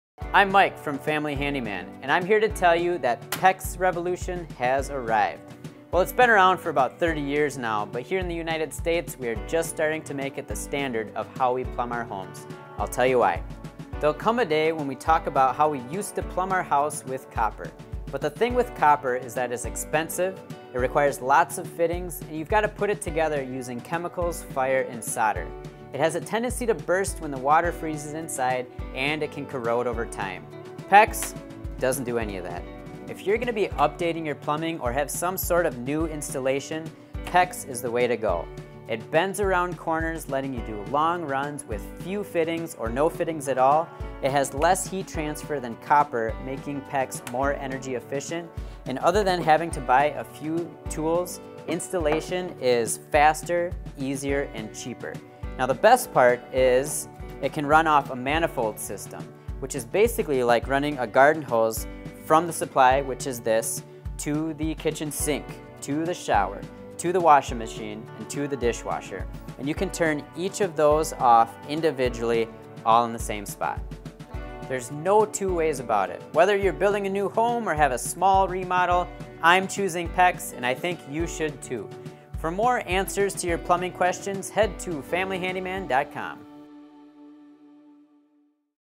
What You Need to Know About PEX Pipe
PEX supply pipe is the biggest revolution in plumbing since the flush toilet, and in this article we’ll answer the most common questions homeowners have about it and also give you some tips for working with it.